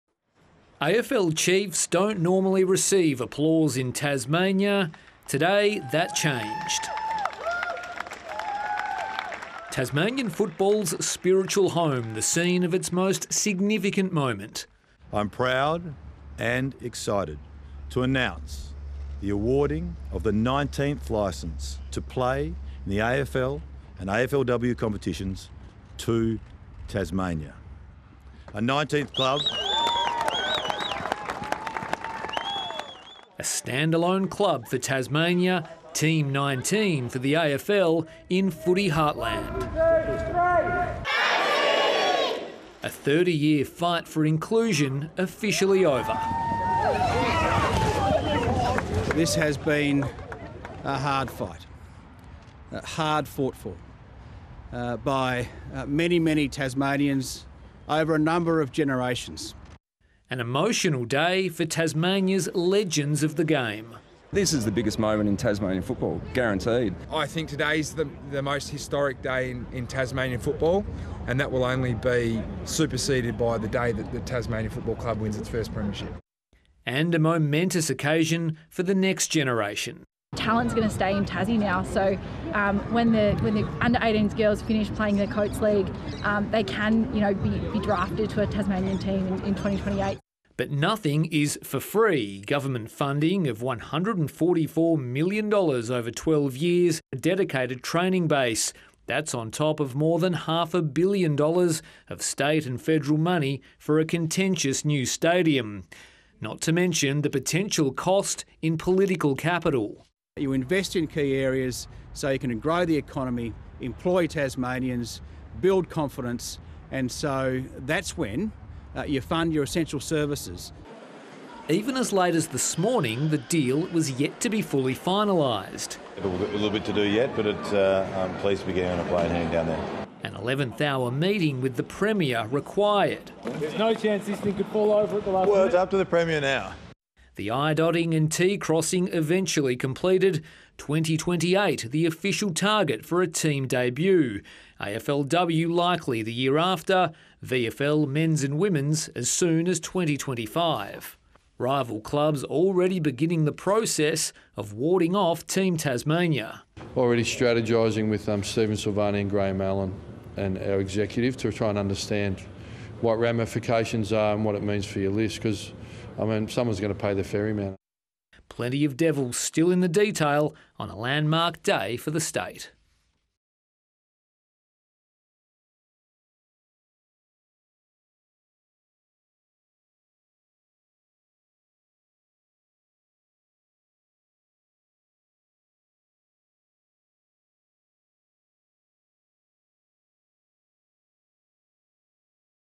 Tasmanian AFL team now a reality after deal signed | ABC News
Tasmania's time "has finally come", Premier Jeremy Rockliff says, after the AFL officially awarded the state the league's 19th team licence.

AFL boss Gillon McLachlan, Acting Prime Minister Richard Marles and other game dignitaries flew into Hobart on Wednesday for the announcement.

"Yesterday afternoon at the meeting of the AFL Commission, where all commissioners were present, the AFL Commission resolved to award a licence based on conditions to a Tasmanian club, a decision that will finally make our national competition truly national, McLachlan announced at North Hobart Oval.